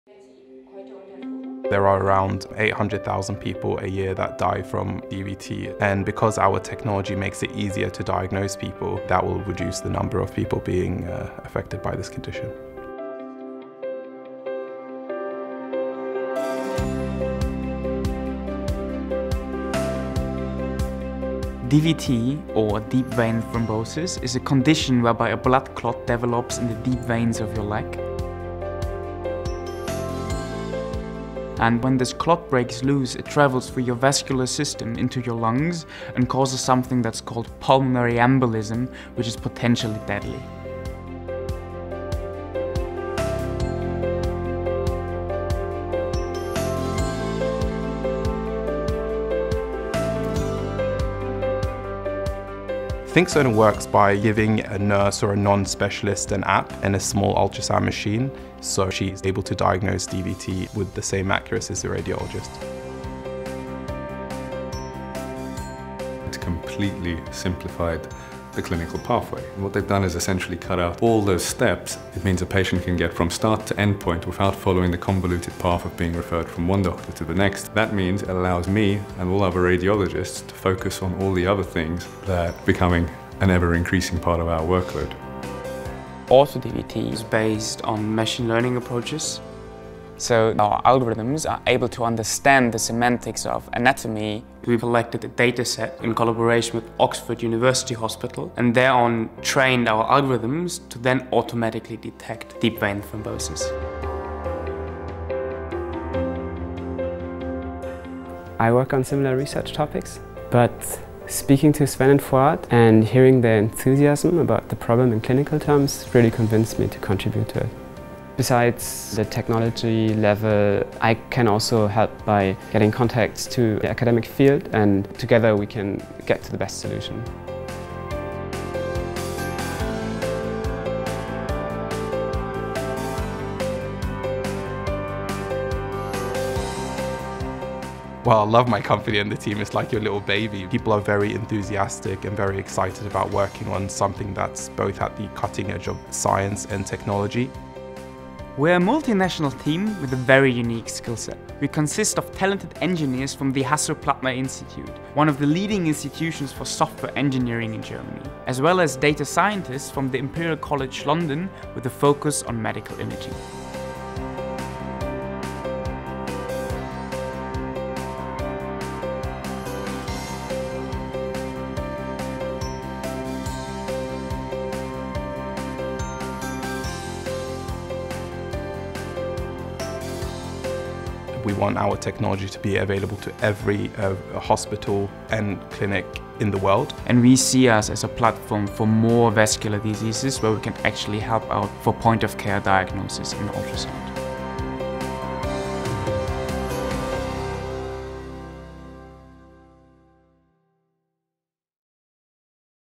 HPI-Startup ThinkSono: Efficient diagnosis of deep vein thrombosis
HPI alumnus Sven Mischkewitz graduated from HPI in 2016 and founded the startup ThinkSono together with Fouad Al-Noor in the same year. The company’s aim is to make the diagnosis of deep vein thrombosis more efficient and to prevent more pulmonary embolisms.

Learn more about the ThinkSono in the video.